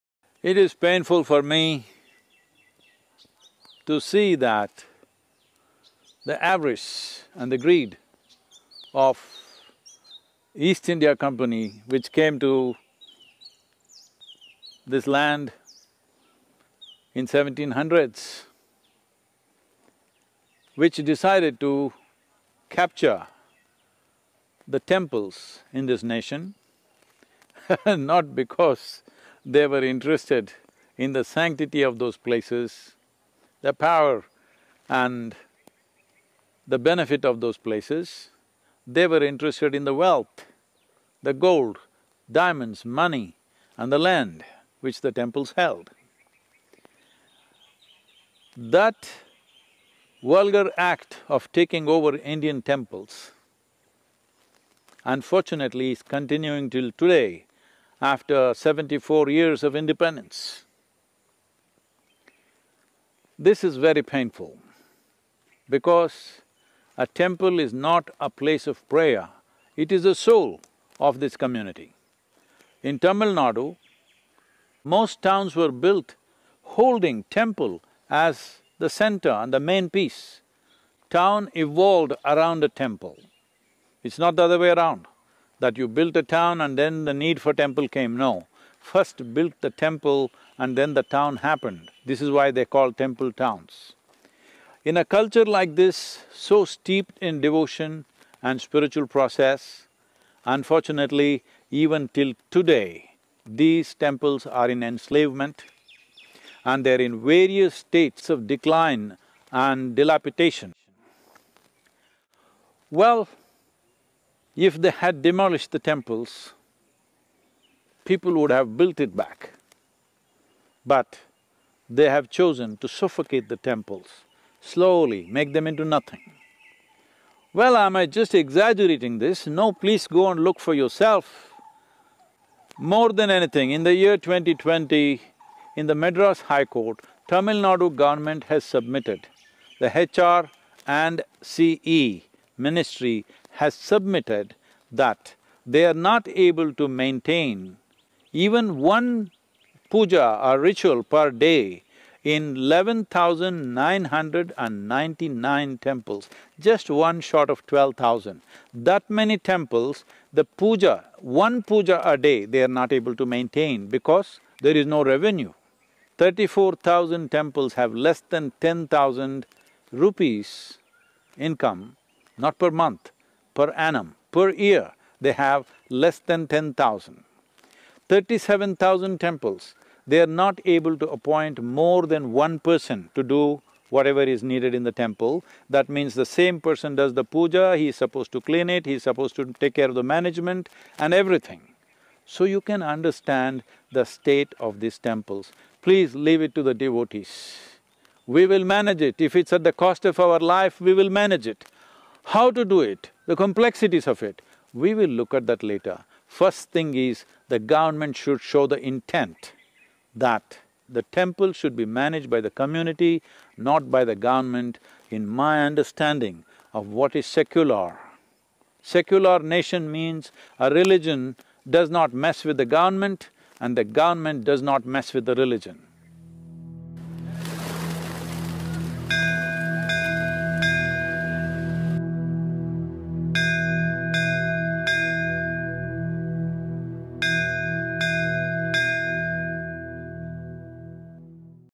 FreeTNTemples | Sadhguru | Free Tamilnadu Temples
Temples are the soul of Tamil community in Tamil Nadu. Towns were built with Temple as the main feature. Today, we have Sarkari souls, systematic suffocation. FreeTNTemples. –Sg